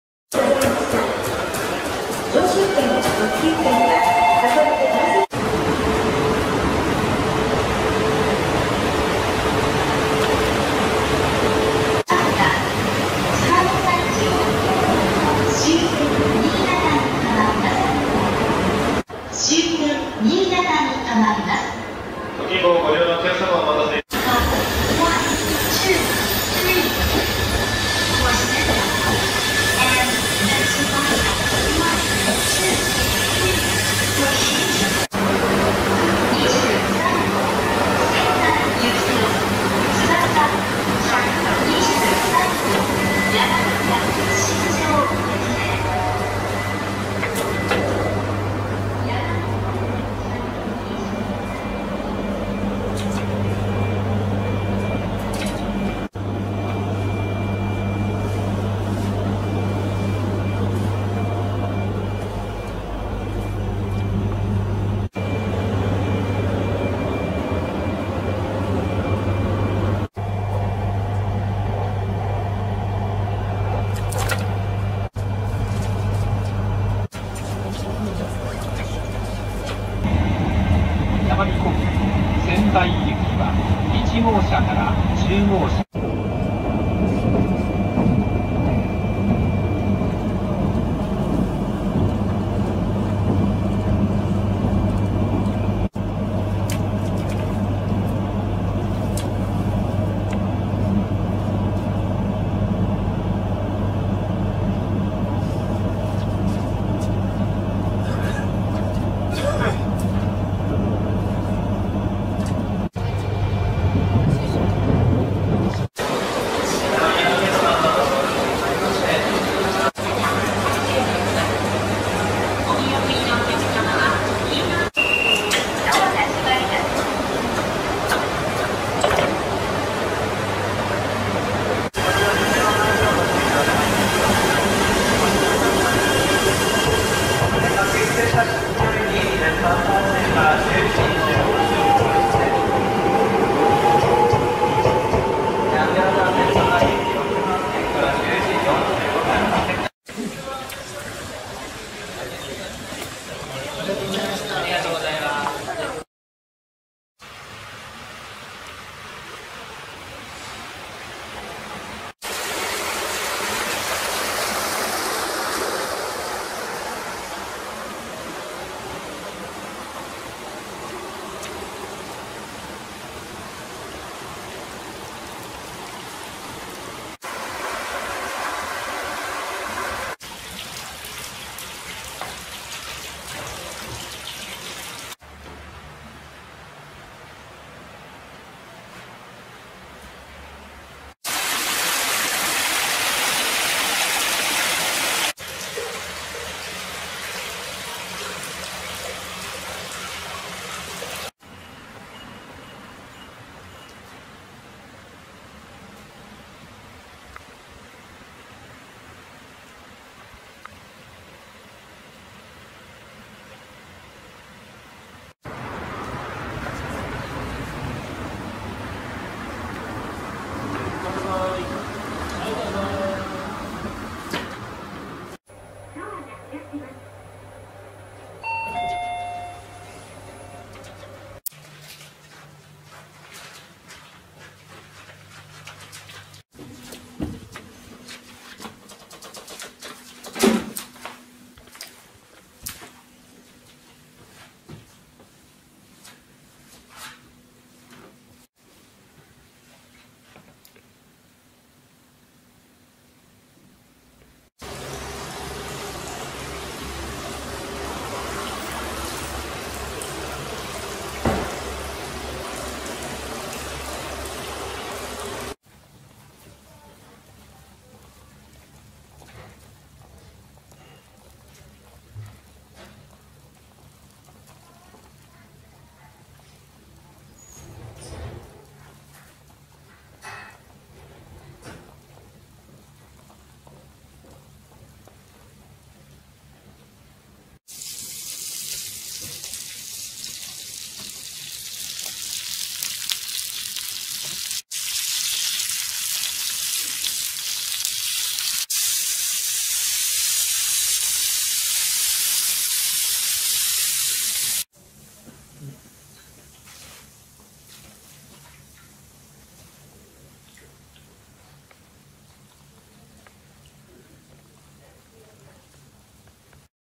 山形日和「蔵王温泉五感の湯つるや」JRびゅう温泉ツアーレポート
JR東日本びゅう 温泉がいい値！山形蔵王温泉五感の湯つるや
山形日和「山形ディスティネーションキャンペーン」
往復限定「つばさ」新幹線利用 温泉旅館1泊2食限定(格安18,800円～)温泉ツアーレポート